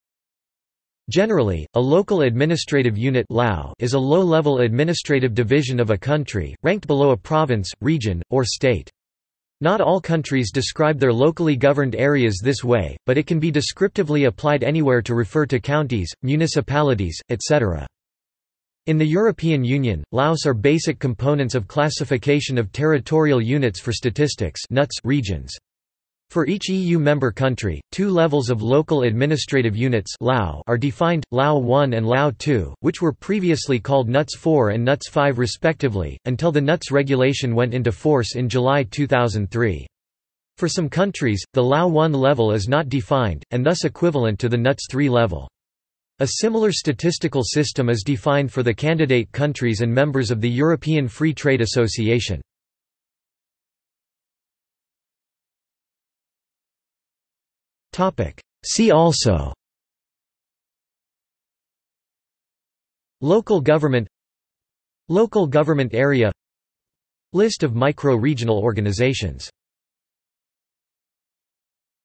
Local administrative unit | Wikipedia audio article
This is an audio version of the Wikipedia Article:
Local administrative unit

00:00:54 undefined

- Socrates

SUMMARY
Generally, a local administrative unit (LAU) is a low level administrative division of a country, ranked below a province, region, or state.  Not all countries describe their locally governed areas this way, but it can be descriptively applied anywhere to refer to counties, municipalities, etc.
In the European Union, LAUs are basic components of Classification of Territorial Units for Statistics (NUTS) regions. For each EU member country, two levels of Local Administrative Units (LAU) are defined: LAU 1 and LAU 2, which were previously called NUTS 4 and NUTS 5 respectively, until the NUTS regulation went into force in July 2003. For some countries, the LAU 1 level is not defined, and thus equivalent to the NUTS 3 level. A similar statistical system is defined for the candidate countries and members of the European Free Trade Association.